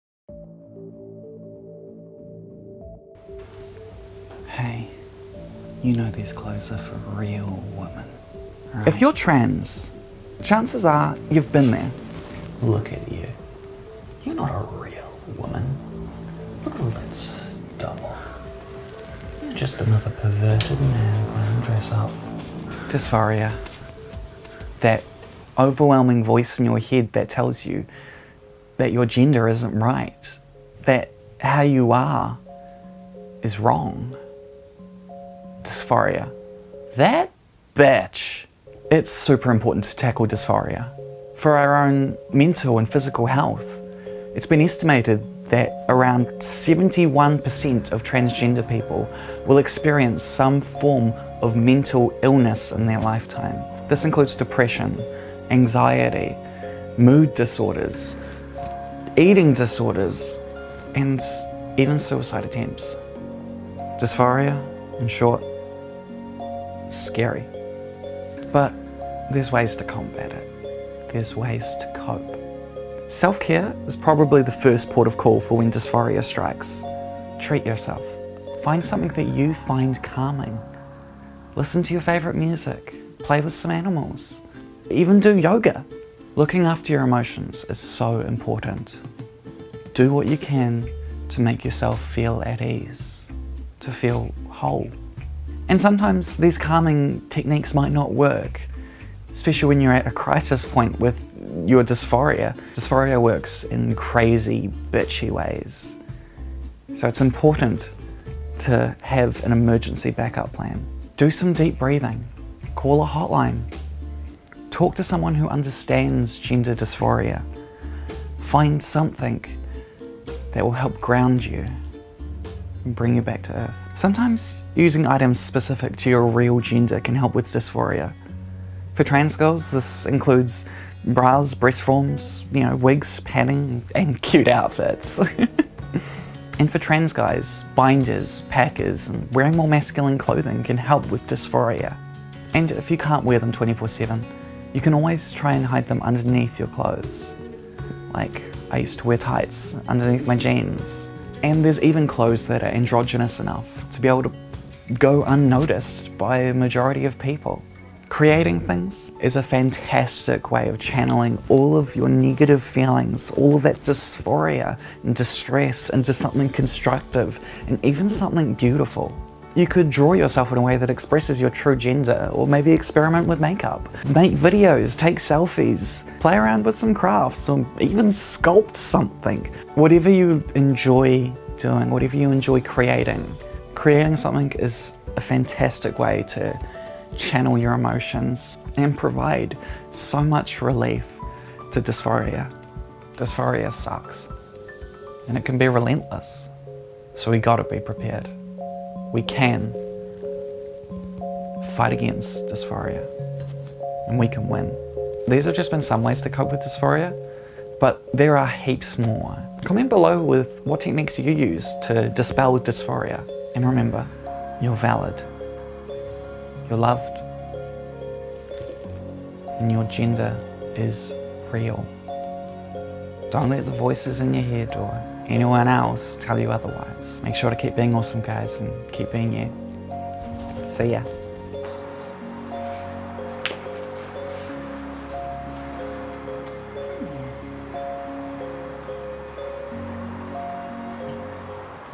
How to Cope with Gender Dysphoria
So let's learn some ways to cope with that dick-bucket known as Gender Dysphoria!